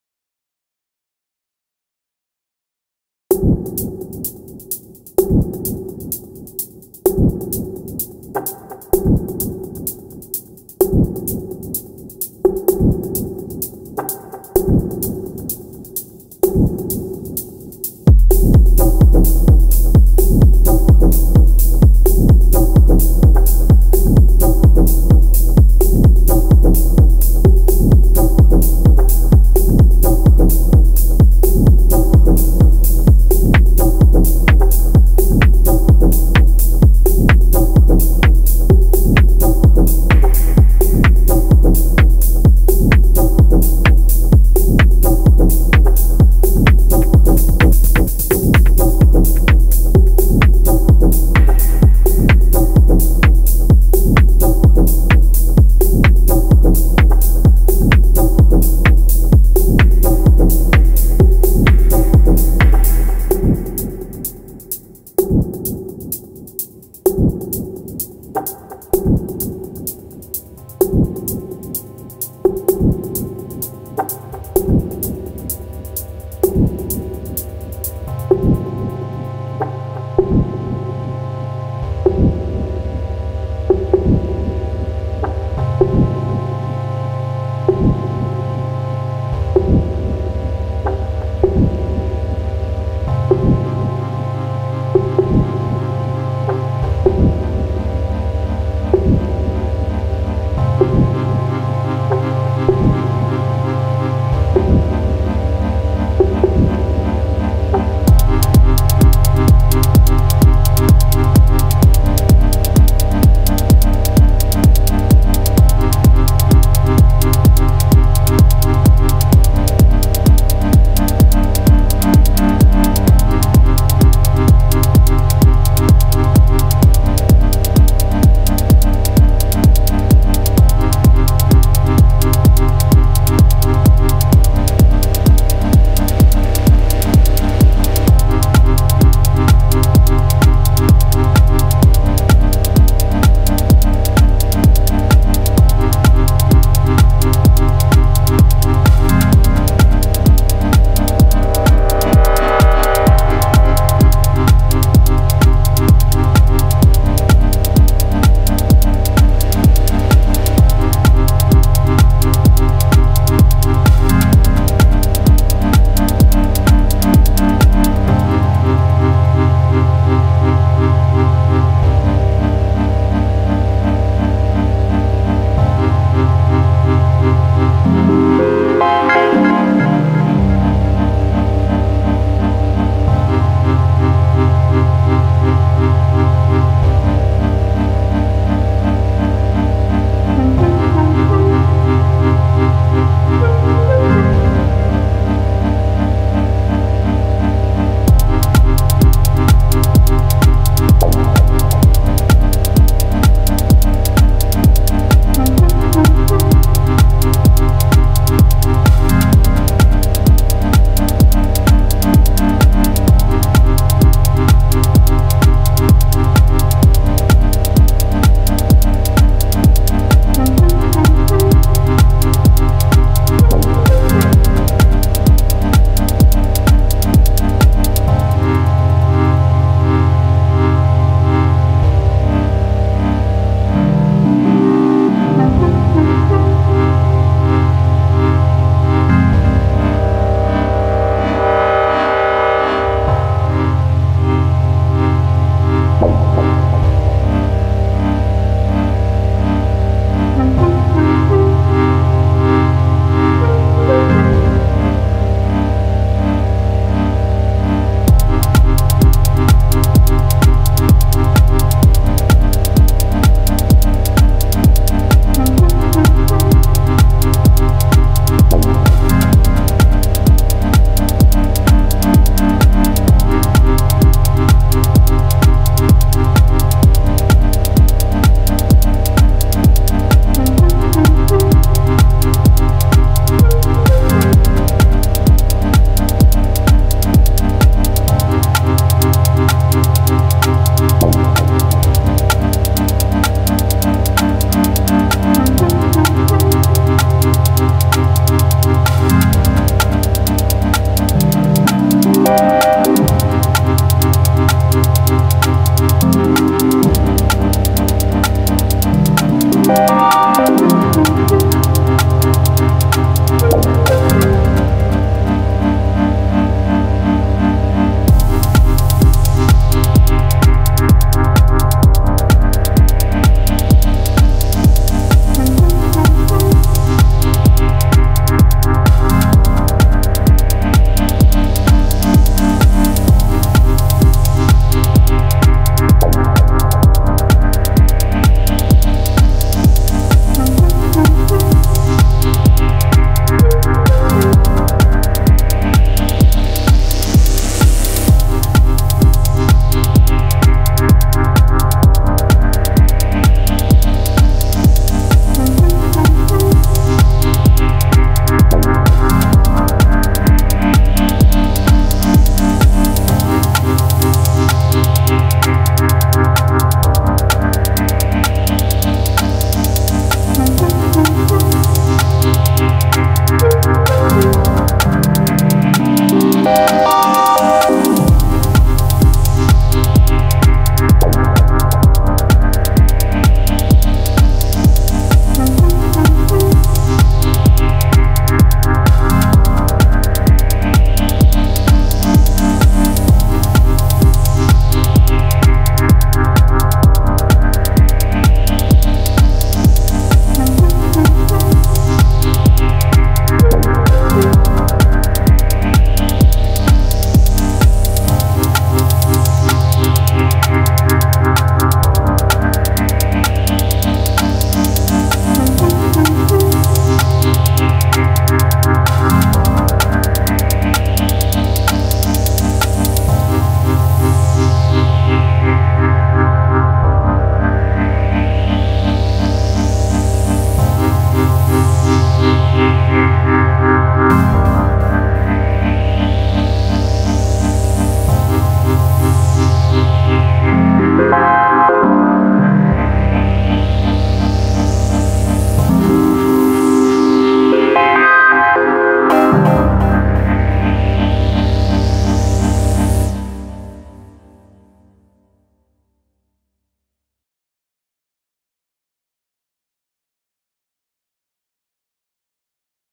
Studio Session ► Digitone Only
I think with enough effort and desire, it's possible to make a whole LIVE set on this thing :D